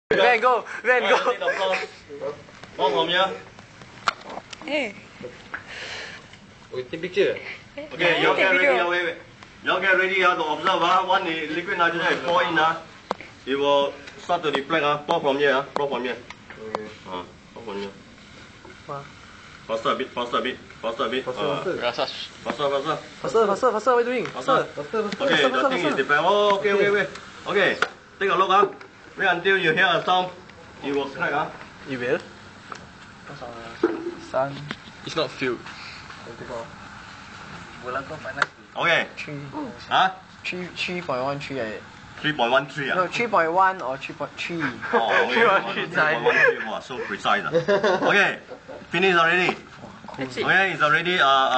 AMP2 practical part 1
We were experimenting with liquid Nitrogen and stuff.
XD